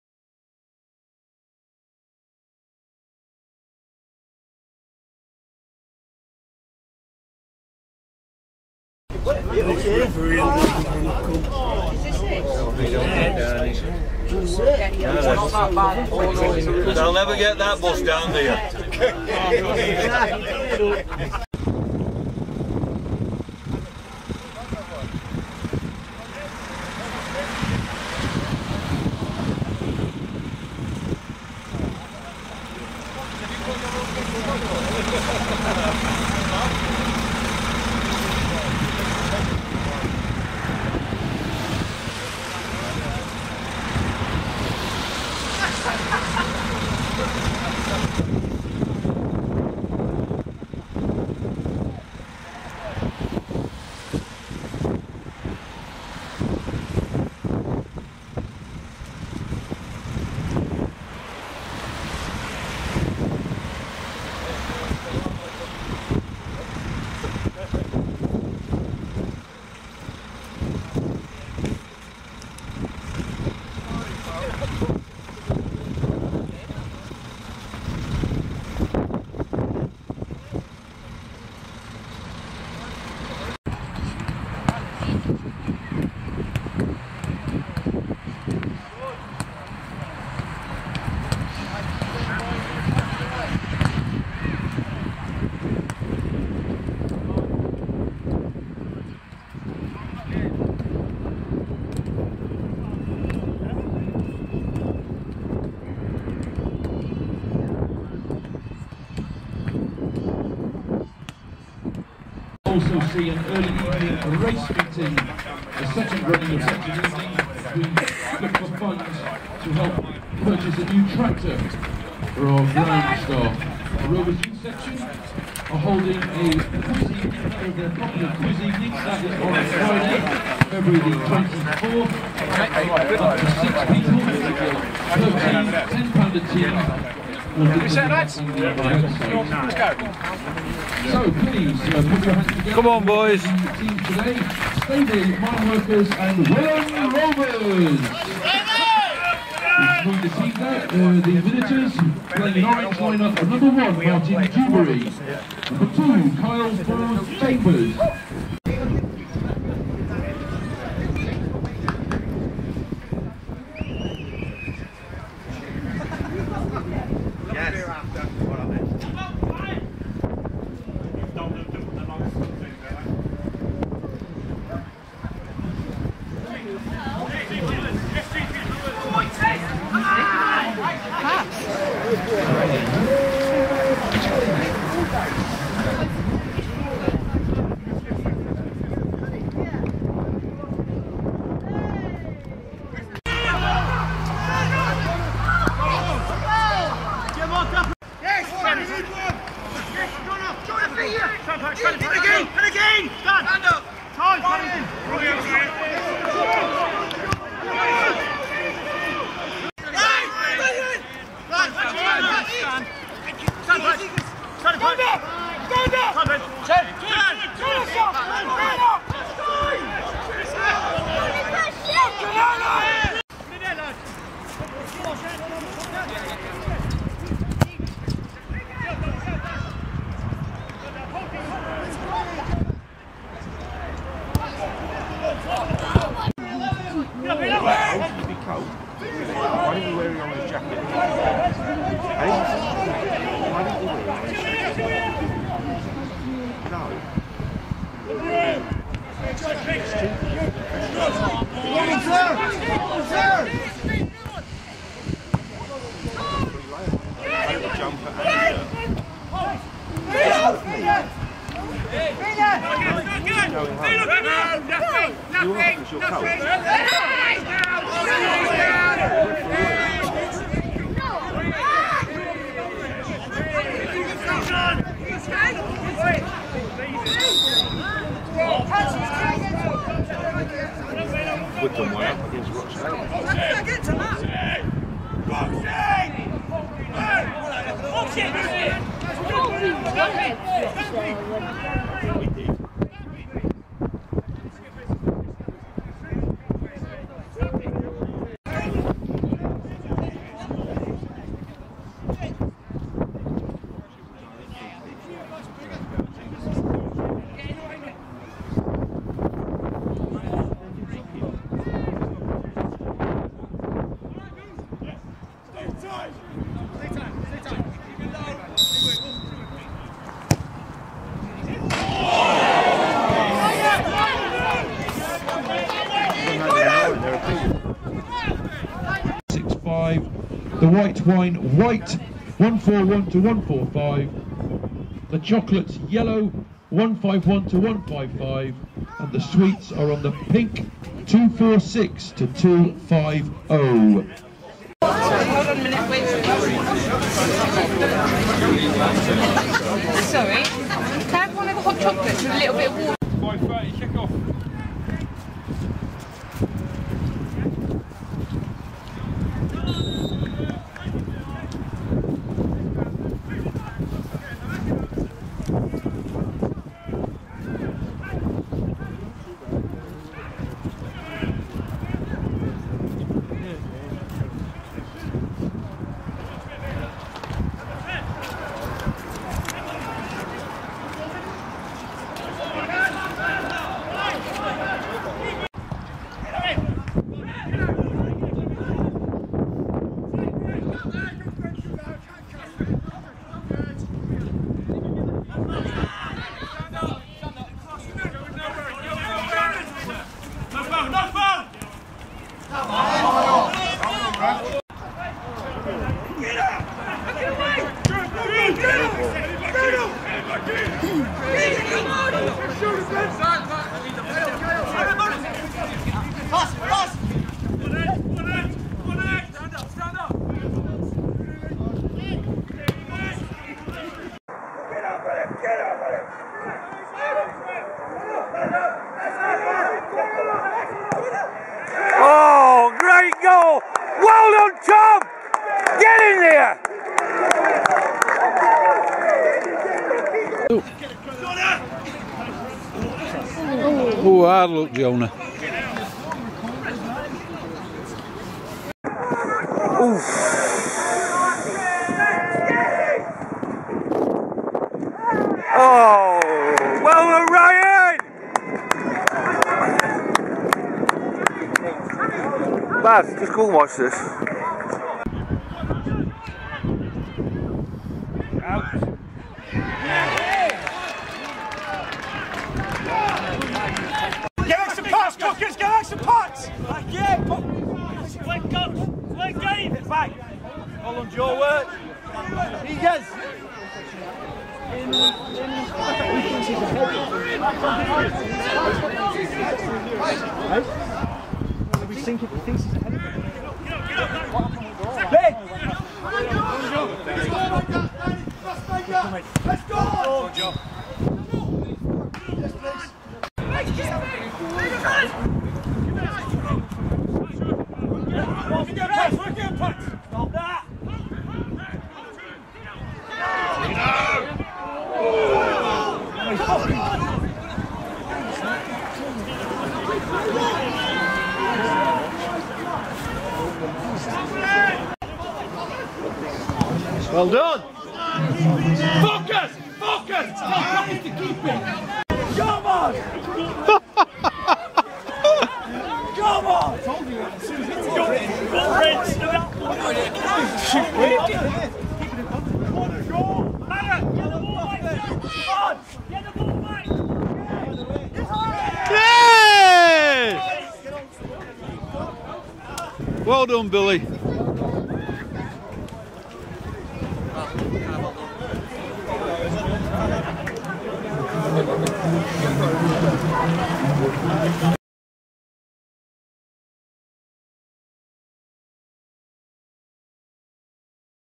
WILLAND ROVERS 1 STAVELEY MWFC 3
Barry R Dyke created this video with the YouTube Video Editor with the goals by Barry Dyke useing a Sony HX5 and the rest of the film recorded with my Canon SX40